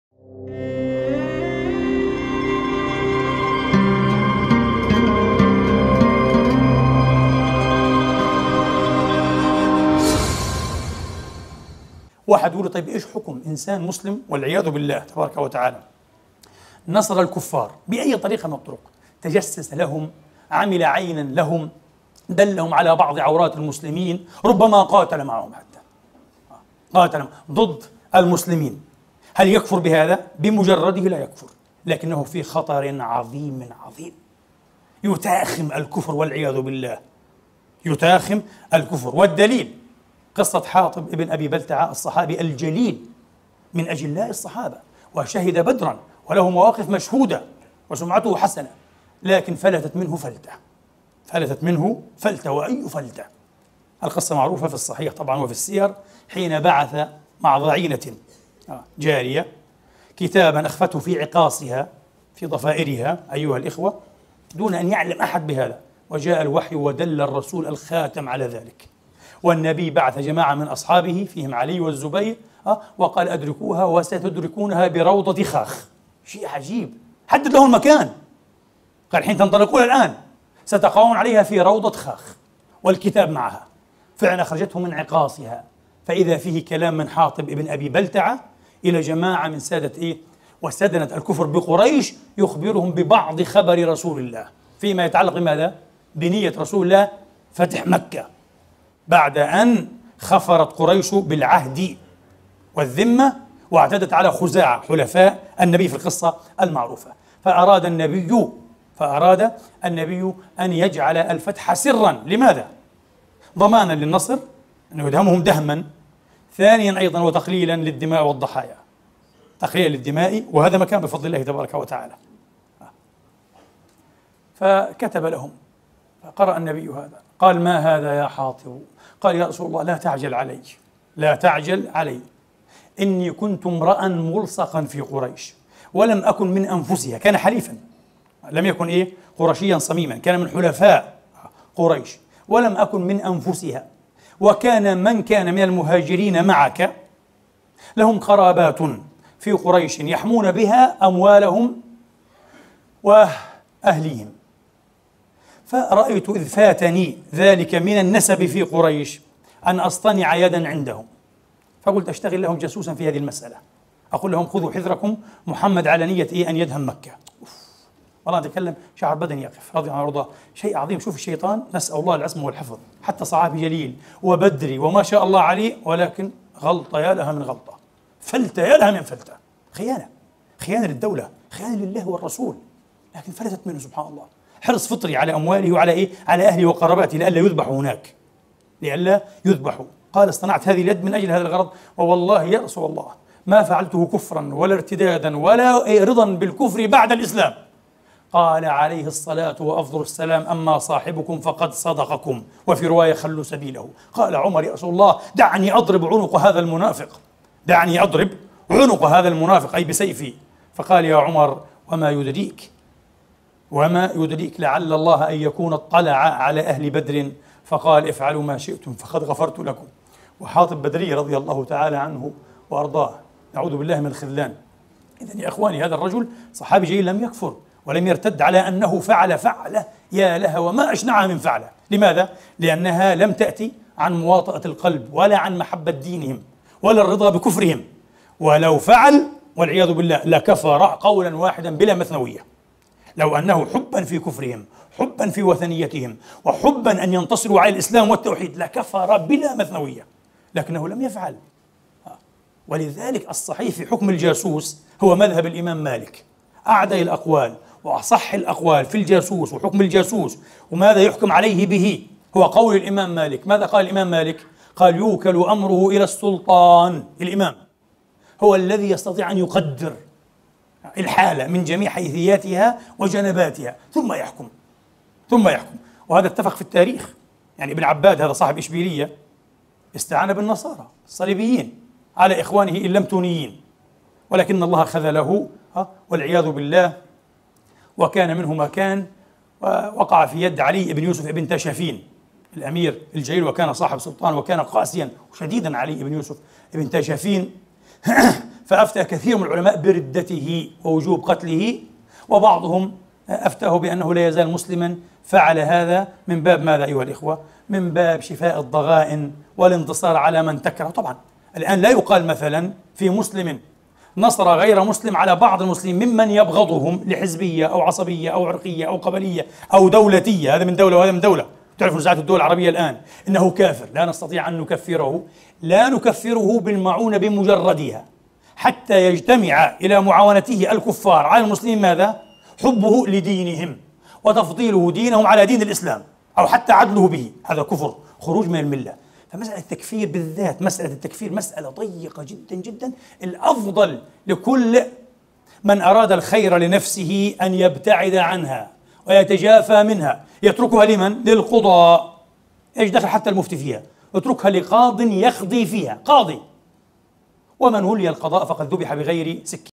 ما حكم من ناصر الكفار و عمل جاسوس لهم - احد الصحابة كمثال ۞ د.عدنان ابراهيم ۞
من خطبة حقيقة الولاء والبراء